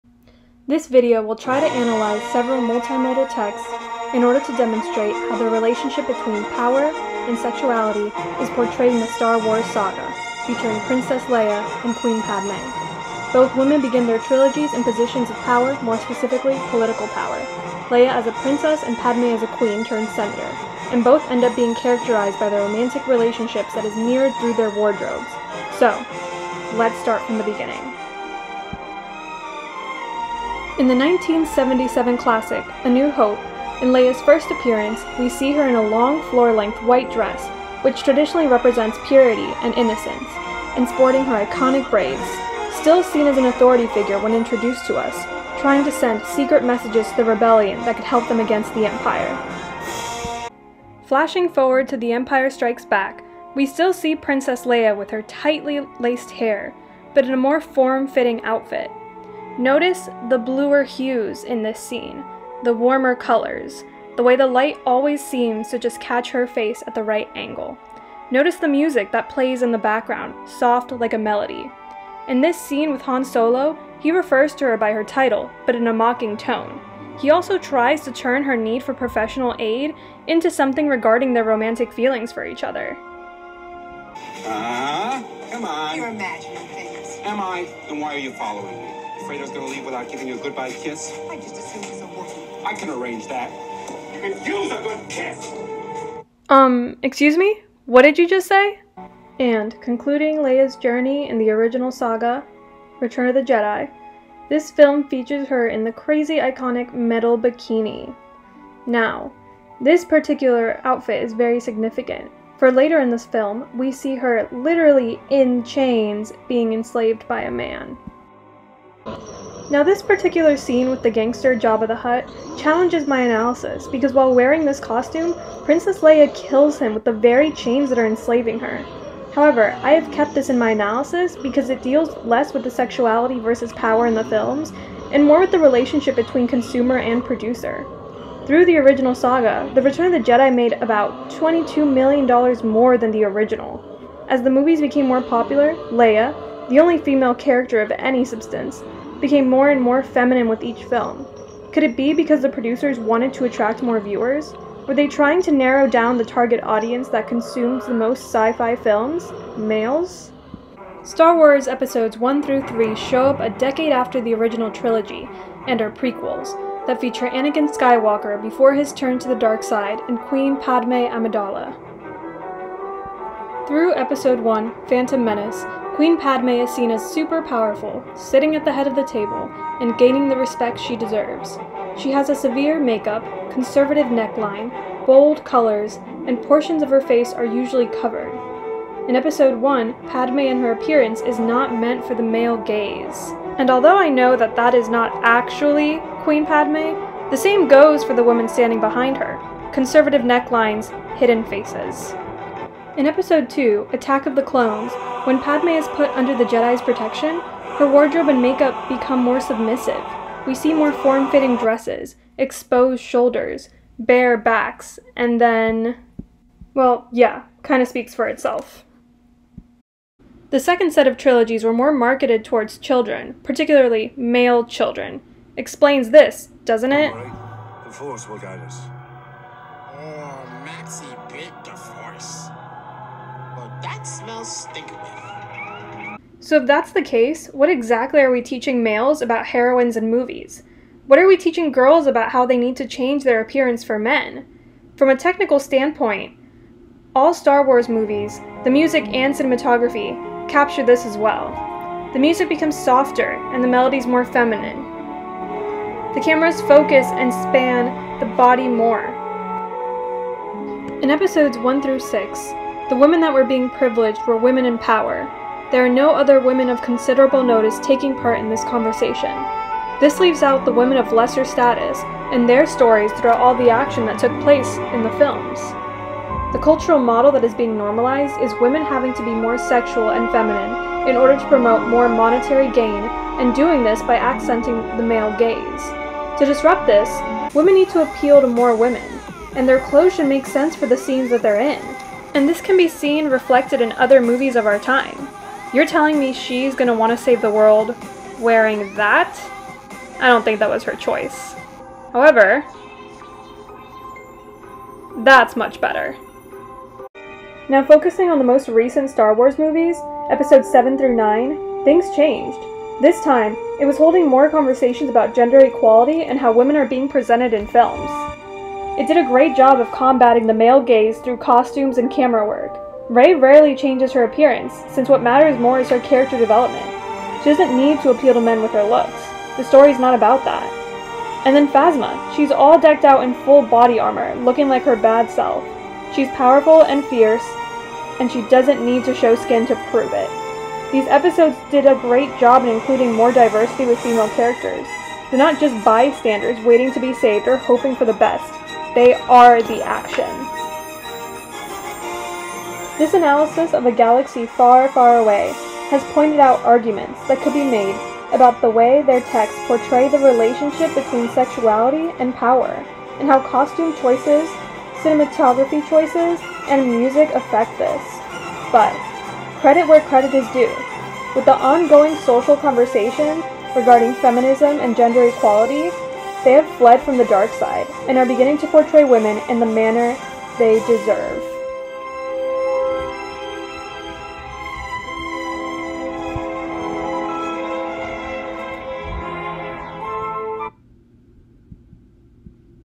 Critical Multimodal Analysis
This video is about Critical Multimodal Analysis that deals with the representation of female characters in the Star Wars saga, focusing on the relationship between power and sexuality which is observed through the development of costume, cinematography, and music.